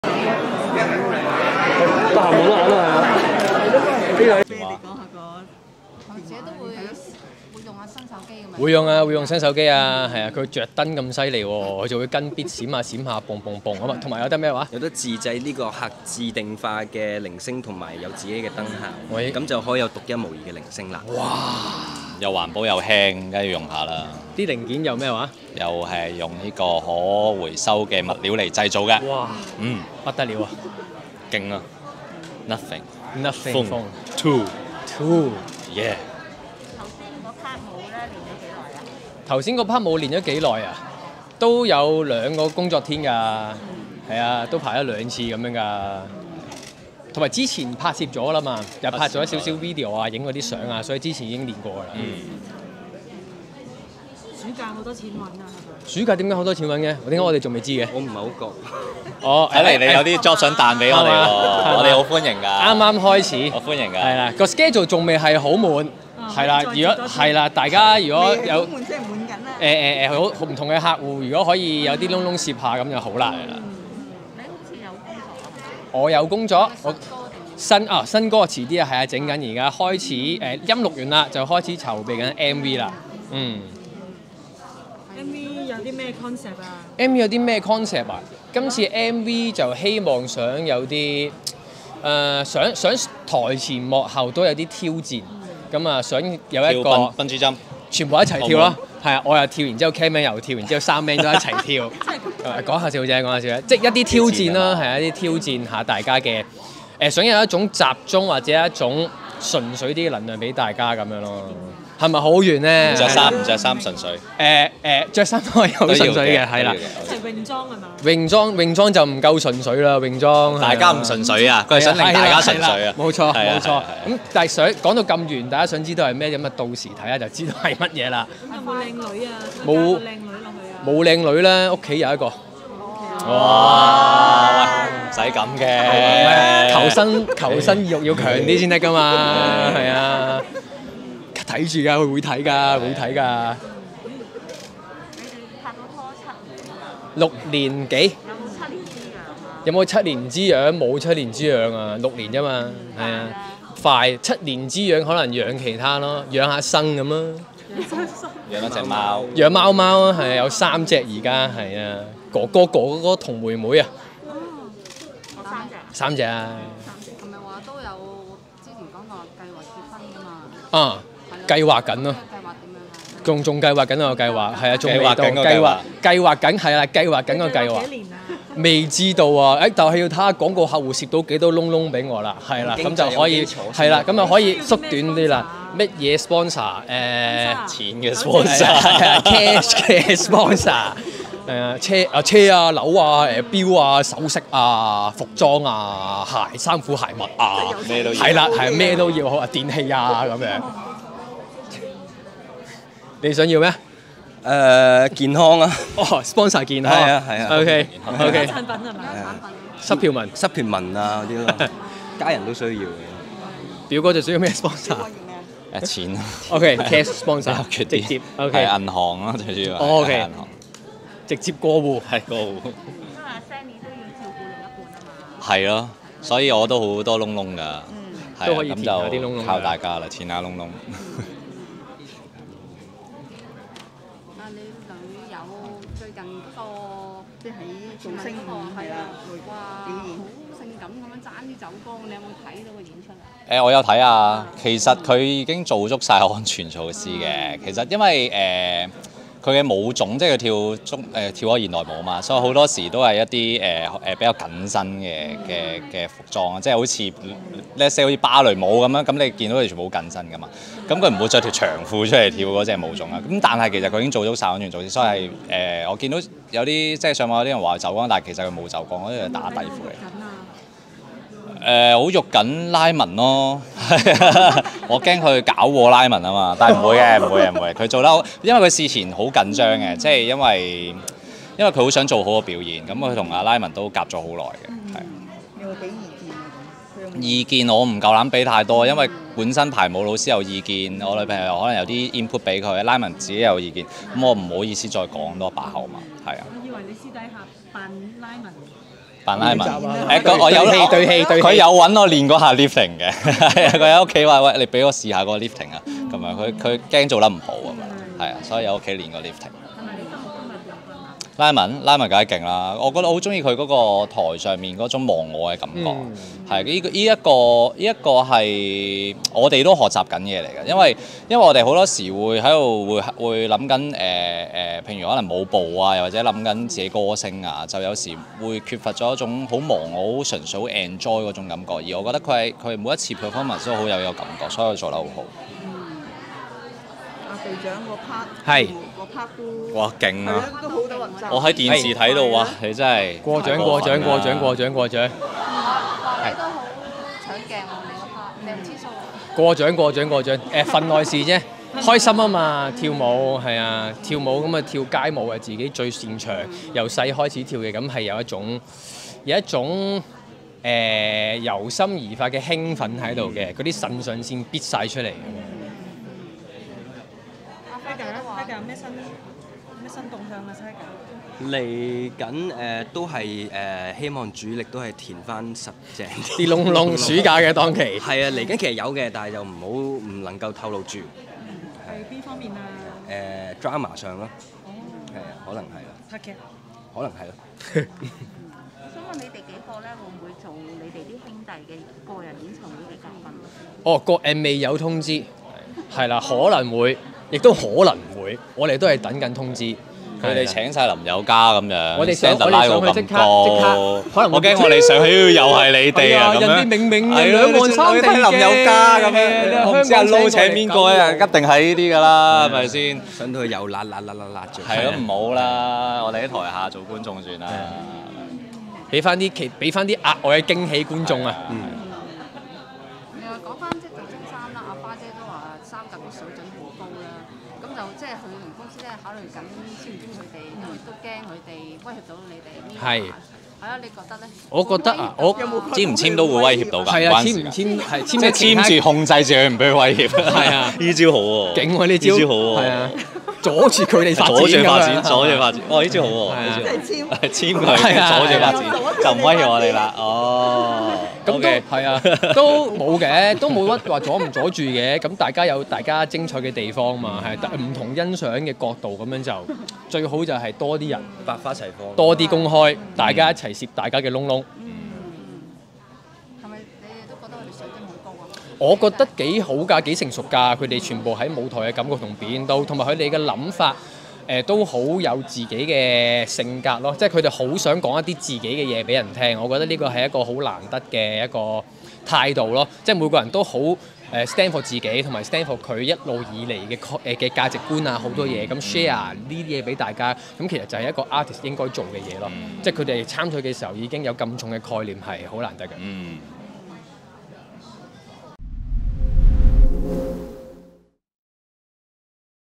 楊樂文提感情事求生欲強 邱士縉 邱傲然對《造星V》三甲威脅有一建議
MIRROR三子Lokman、Stanley、Tiger今日（19日）出席新手機發布會，問到暑假是否多賺錢工作，Lokman卻指未算排滿，但已錄起新歌準備拍MV，他笑言MV無靚女，只有屋企一個靚女，更指自己求生意欲好強。問到跟女友拍拖逾六年會否七年之癢，他笑言未癢，只養了三隻貓，結婚仍在計劃，視乎有幾多工作，希望有贊助，還即場搲撈車、樓、電器、服裝無任歡迎。Tiger最想有健康產品贊助，Stanley直言想要現金。